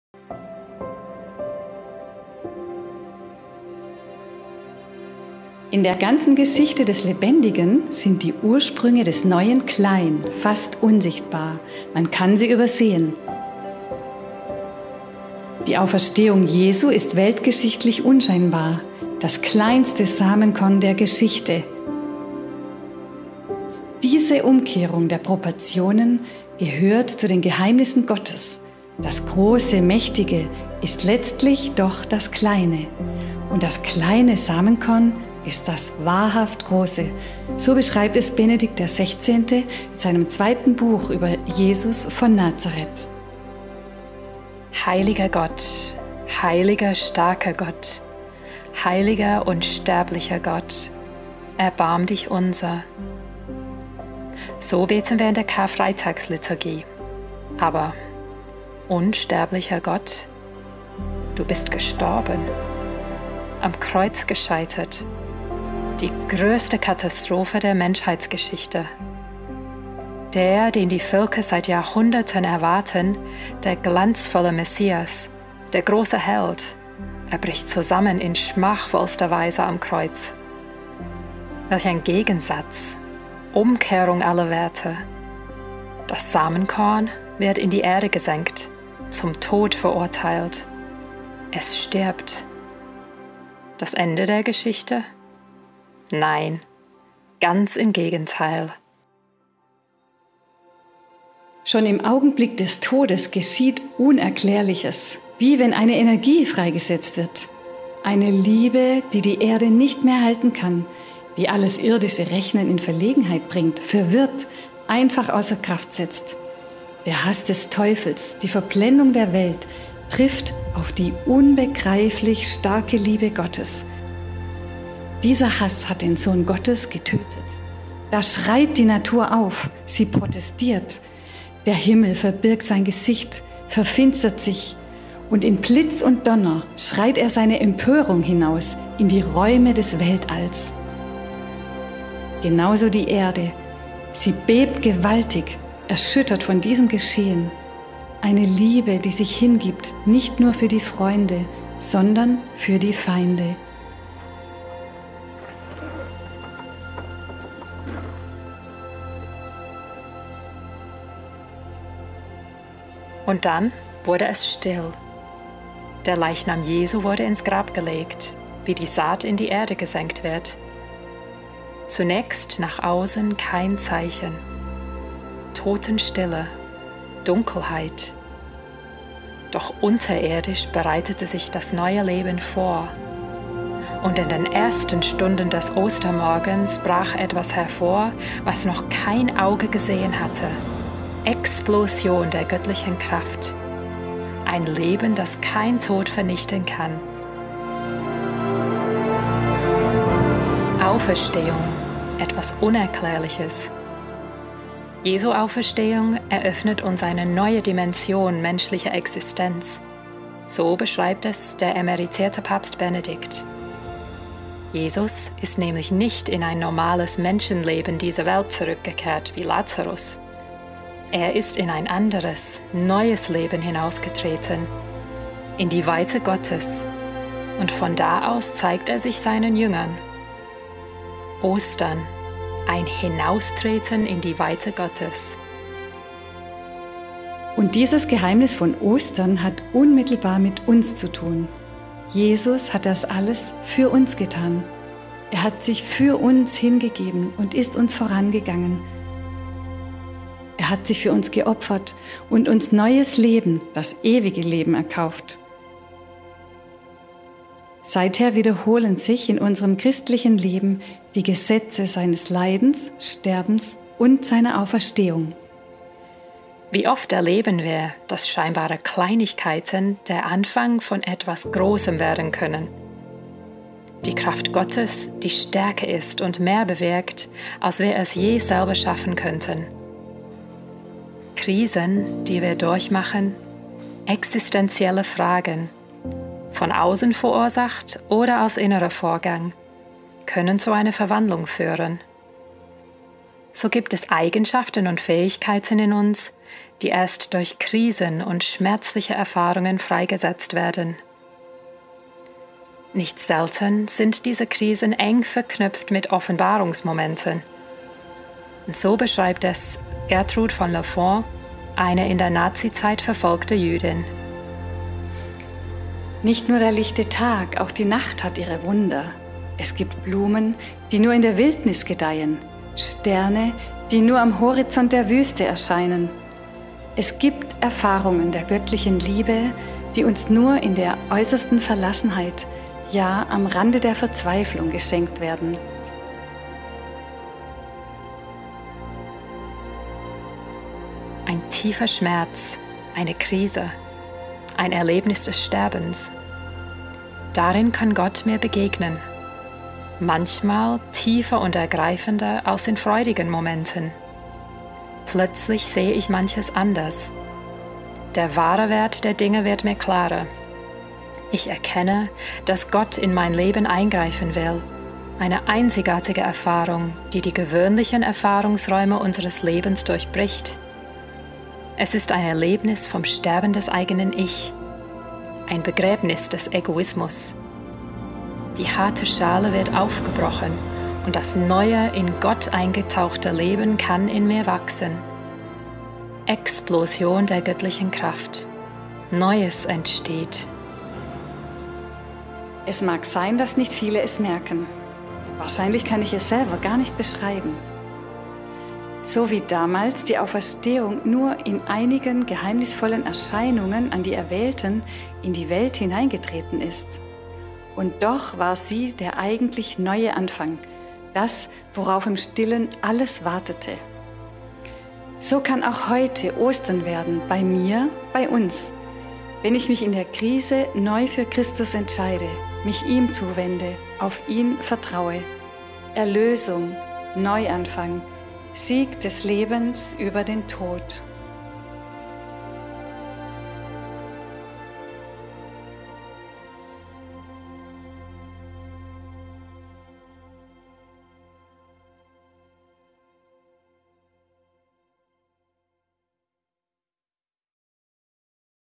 Ostern: das Große fängt klein an /Easter: the great begins small /Pascua: lo grande comienza pequeño
DE: Ostern - zwischen Für-immer-vorbei und Der-Herr-ist-auferstanden. Ein Gehen auf der Grenze. Zwischen Tod und Leben. Oder durch den Tod zum Leben. Es ist wie bei einem kleinen Samenkorn: Wenn es in die Erde fällt und stirbt, bringt es reiche Frucht. Umkehrung aller Werte: das Kleine ist das eigentlich Große. Und das Große, Mächtige ist letztlich doch das Kleine. Das Osterfest, die Auferstehung Jesu ist ein Geheimnis, dem wir uns in diesem Video nähern wollen.

ES: Pascua - entre Forever-Gone y The-Lord-is-Risen. Caminando por la frontera. Entre la muerte y la vida. O a través de la muerte a la vida. Es como una pequeña semilla: cuando cae en la tierra y muere, da abundantes frutos. Inversión de todos los valores: lo pequeño es en realidad lo grande. Y lo grande, lo poderoso es, en última instancia, lo pequeño. La celebración de la Pascua, la Resurrección de Jesús es un misterio que queremos abordar en este vídeo.

EN: Easter - between Forever-Gone and The-Lord-is-Risen. Walking on the border. Between death and life. Or through death to life. It's like a small seed: when it falls into the earth and dies, it bears abundant fruit. Reversal of all values: the small is the actually great. And the great, powerful is ultimately the small. The Easter, the Resurrection of Jesus is a mystery, which we want to approach in this video.

Video credit:  @JenseitsdesGewohnlichen

Stock footage: Videezy.com, Pixabay.com